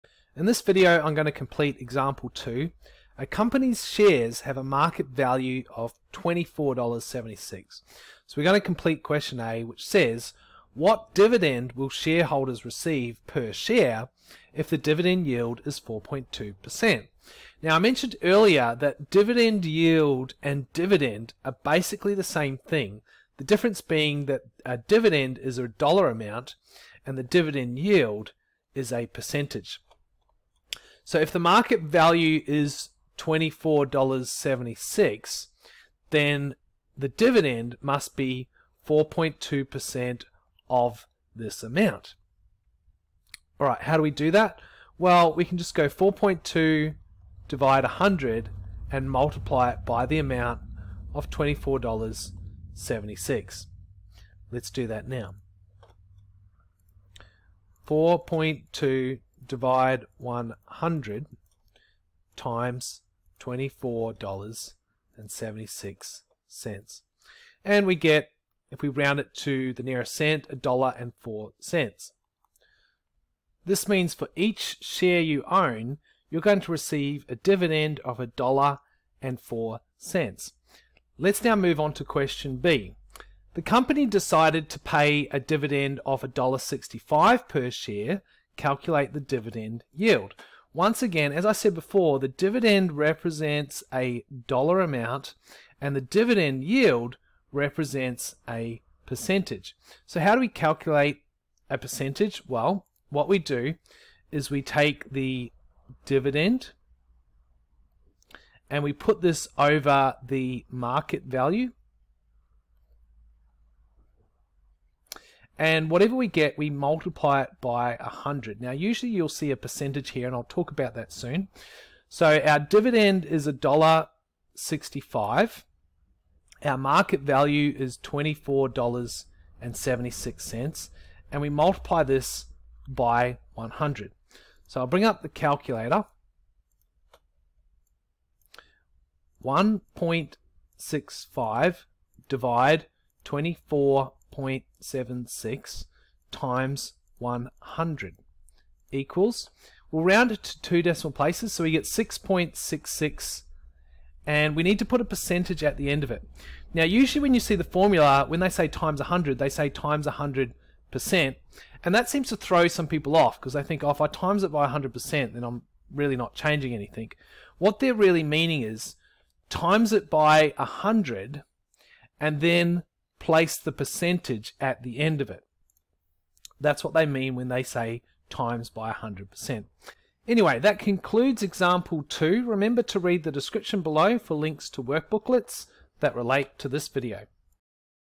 3D Shares and Dividends (3 of 3)
completes an example where you make calculations involving shares and dividends.